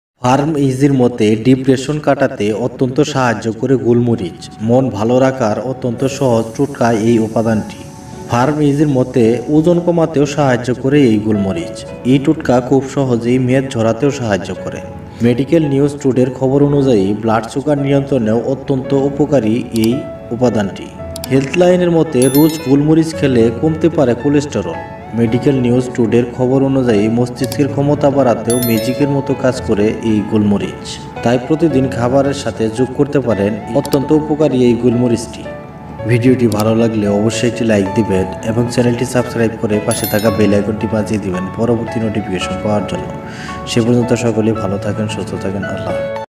বাড়বে মস্তিষ্কের ক্ষমতা! চিরকালের মতো দূর হবে কোলেস্টেরল, রোজ খান এই উপাদান!
ফার্ম ইজির মতে ডিপ্রেশেন কাটাতে অত্যন্ত সাহায্য করে গোল মরিচ। মন ভাল রাখার অত্যন্ত সহজ টোটকা এই উপাদান।

ফার্ম ইজির মতে, ওজন কমাতেও সাহায্য করে গোল মরিচ। এই টোটকা খুব সহজেই মেদ ঝরাতে সাহায্য করে।

মেডিক্যাল নিউজ টুডের খবর অনুযায়ী ব্লাড সুগার নিয়ন্ত্রণেও অত্যন্ত সাহায্য করে এই উপাদান।

হেল্থ লাইনের মতে রোজ গোল মরিচ খেলে কমতে পারে কোলেস্টেরল।

মেডিক্যাল নিউজ টুডের খবর অনুযায়ী মস্তিষ্কের ক্ষমতা বাড়াতেও ম্যাজিকের মতো কাজ করে গোল মরিচ।